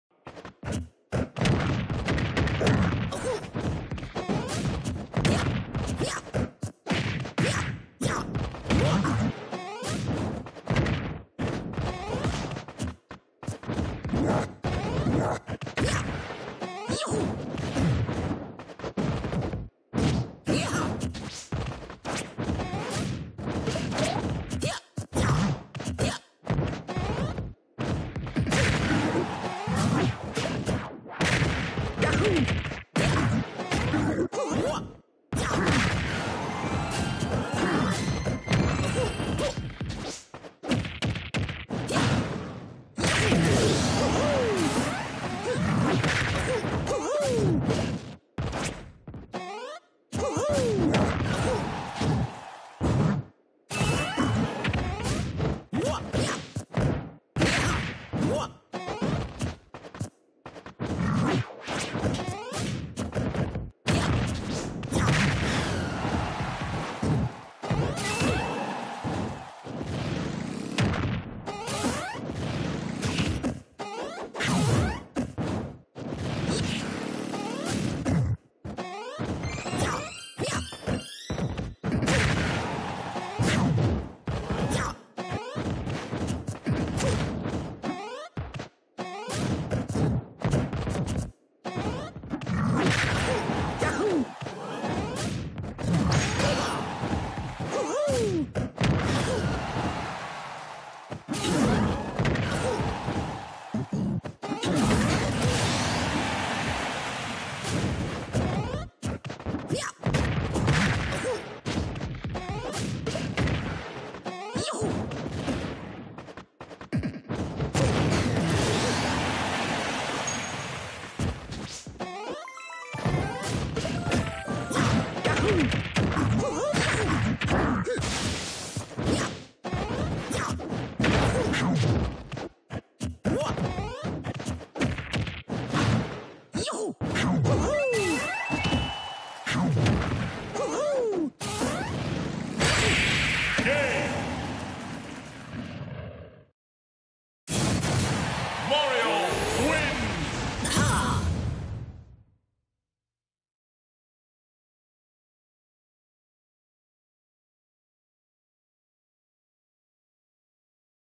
This is the match that got me to 11,400,000
Really got him in the ending. Thinking I should edit these just to cut our boring parts, but sometimes I like leaving the whole match for analysis.